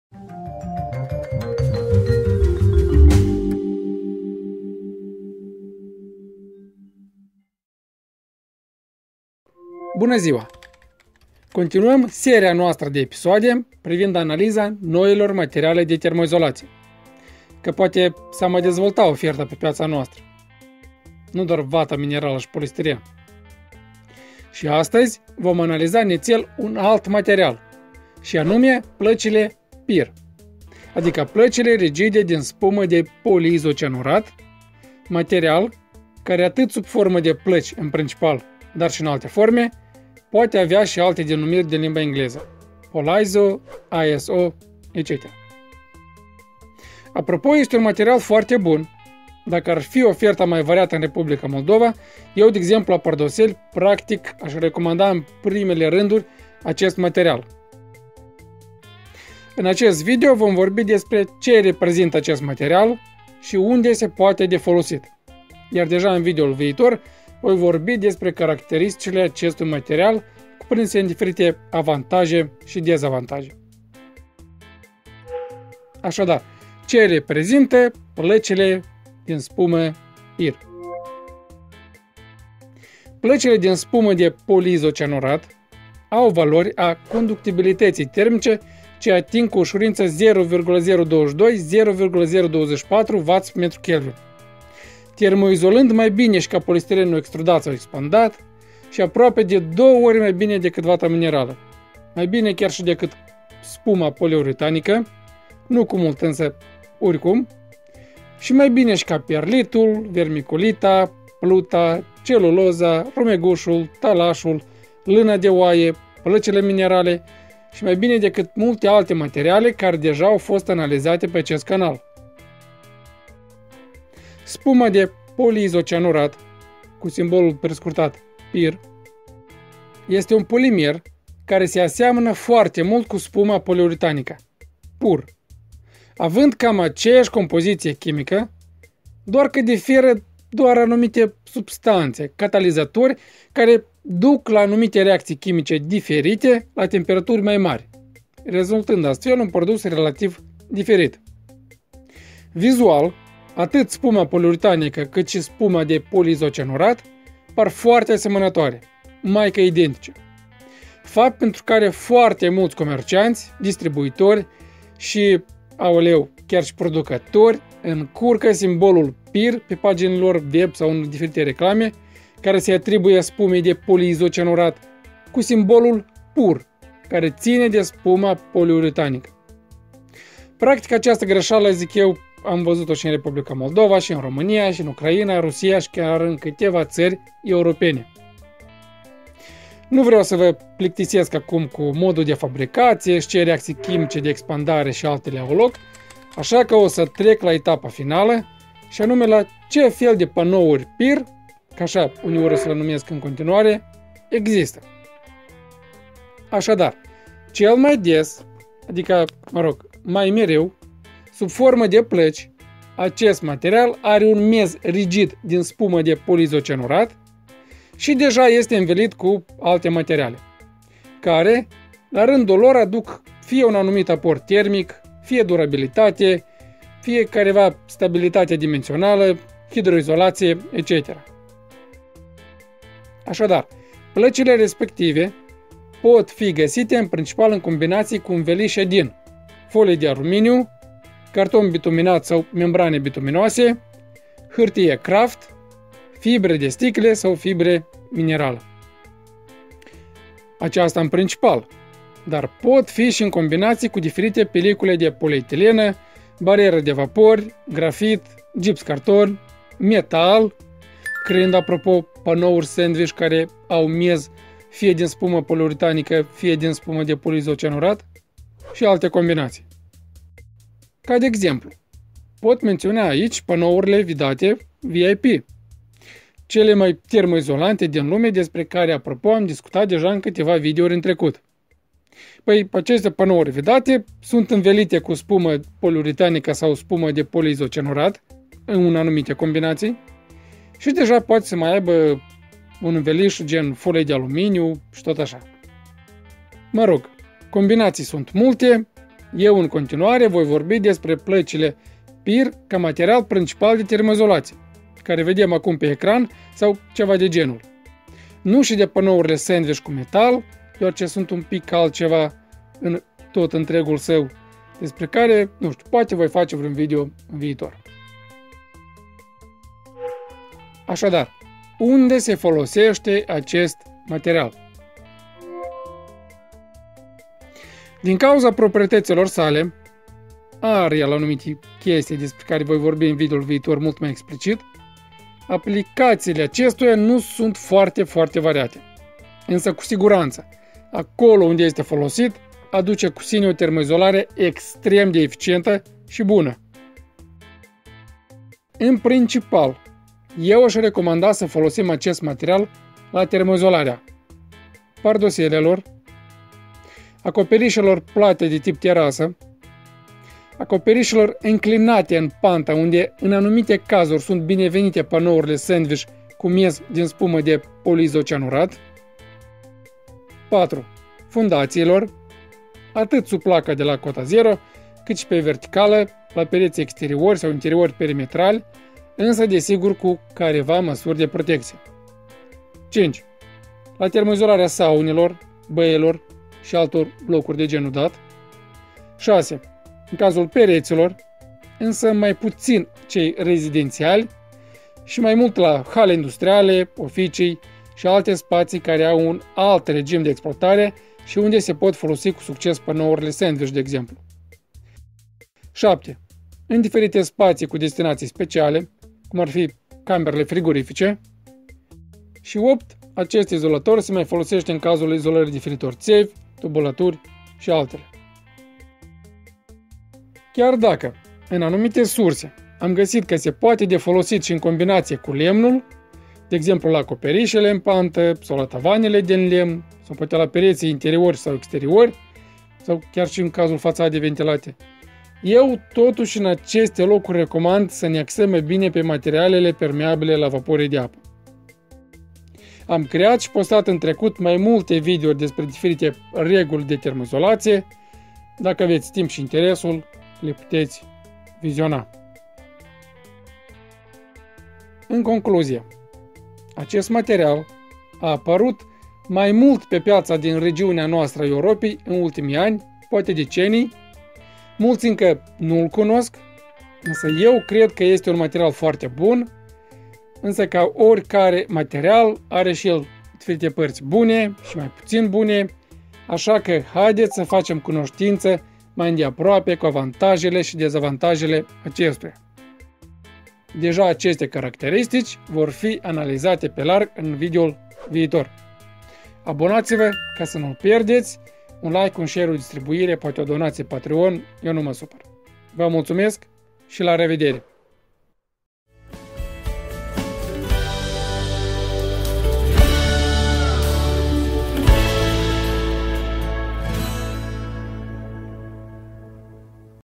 Termoizolatia cu placi PIR (spuma poliizocianurică) – Ce reprezinta si Unde se foloseste
Termoizolatia cu Placile PIR care se gasesc si in Republica Moldova, si Romania si pe scara larga deja in toate tarile europene, reprezinta placi rigide din poliizocianurat, care atinge valori de conductivitate termica chiar de 0.022 W/mK (mult mai bun ca polistirenul, vata minerala si multe alte materiale de izolare termica). Acest video va fi despre ce reprezinta si unde se folosesc placile PIR.

RECTIFICARE: la minutul 01:28 e scris o termoconductibilitate de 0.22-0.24 W/mK - acest lucru e o greseala (recunosc), corect ar fi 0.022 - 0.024 W/mK. Cer scuze, multumesc! Multumesc de vizionare, va doresc o zi frumoasa!